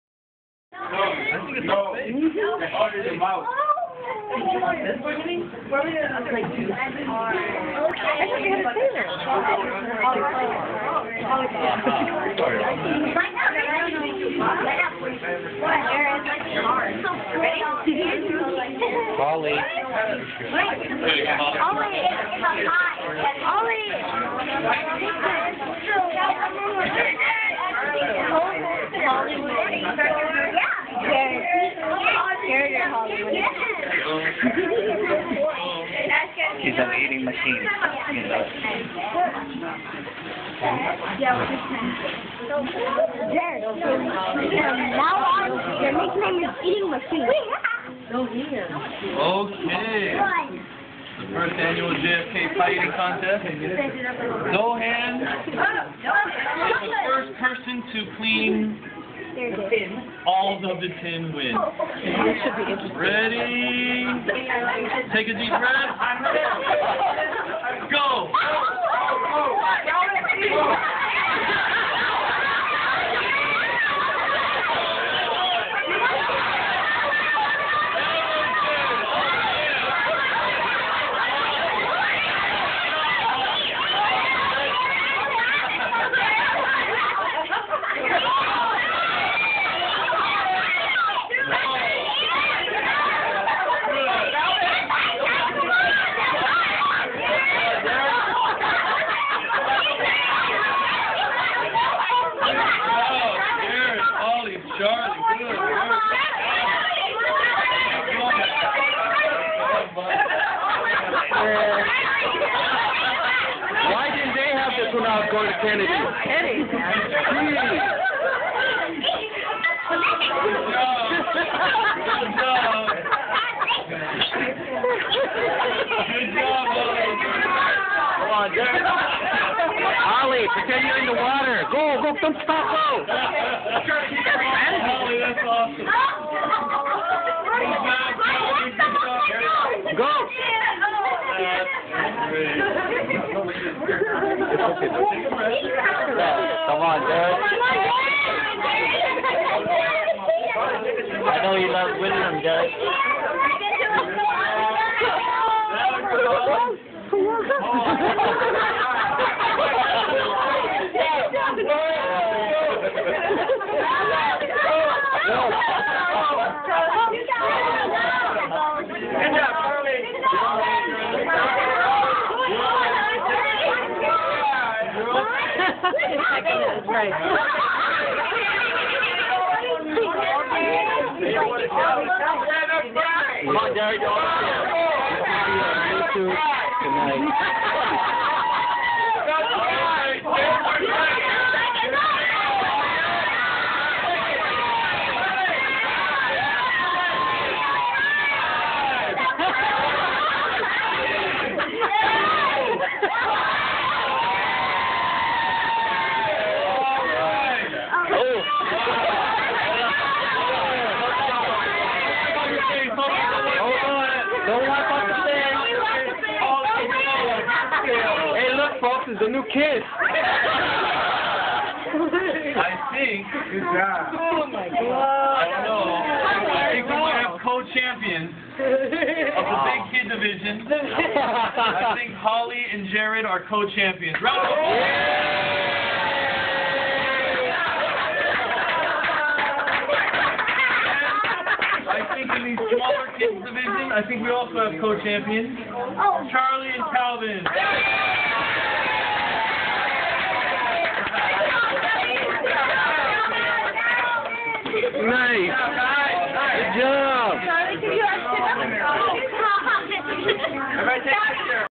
JFK elementary's Pie Eating Contest
Daly City's John F. Kennedy elementary School Fundraiser @ Marie Callendar's